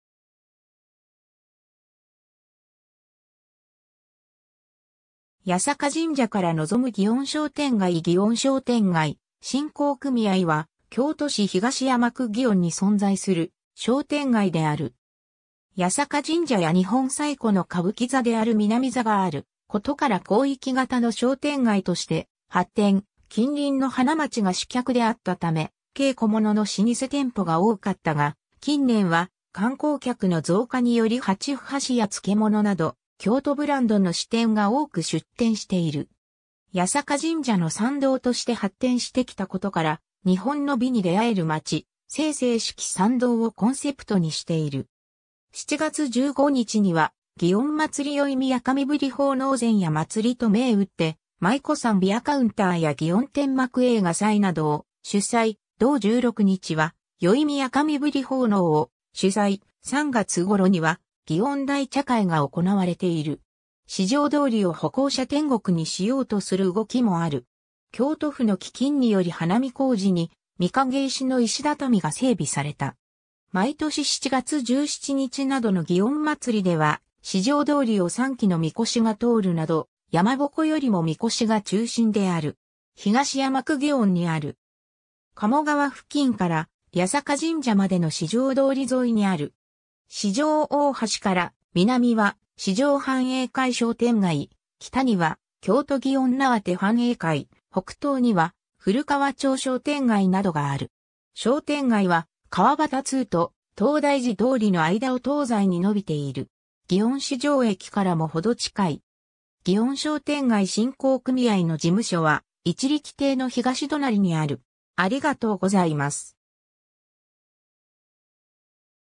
祇園商店街振興組合
八坂神社から望む祇園商店街 祇園商店街振興組合（ぎおんしょうてんがいしんこうくみあい）は、京都市東山区祇園に存在する商店街である。
八坂神社や日本最古の歌舞伎座である南座がある事から広域型の商店街として発展、近隣の花街が主客であった為、京小物の老舗店舗が多かったが、近年は観光客の増加により八ッ橋や漬物など京都ブランドの支店が多く出店している。
　八坂神社の参道として発展してきたことから「日本の美に出会えるまち」「清々しき参道」をコンセプトにしている。
7月15日には祇園祭宵宮神振奉納前夜祭と銘打って舞妓さんビアカウンターや祇園天幕映画祭などを主催、同16日は宵宮神振奉納を主催、3月頃には祇園大茶会が行われている。
四条通りを歩行者天国にしようとする動きもある。
 京都府の基金により花見小路に御影石の石畳が整備された。
 毎年7月17日などの祇園祭りでは、四条通りを3基の神輿が通るなど、山鉾よりも神輿が中心である。
 東山区祇園にある。
鴨川付近から八坂神社までの四条通り沿いにある。
四条大橋から南は四条繁栄会商店街、北には京都祇園縄手繁栄会、北東には古川町商店街などがある。
 商店街は川端通と東大路通の間を東西に伸びている。
 祇園四条駅（京阪本線）からもほど近い。
 祇園商店街振興組合の事務所は一力亭の東隣にある。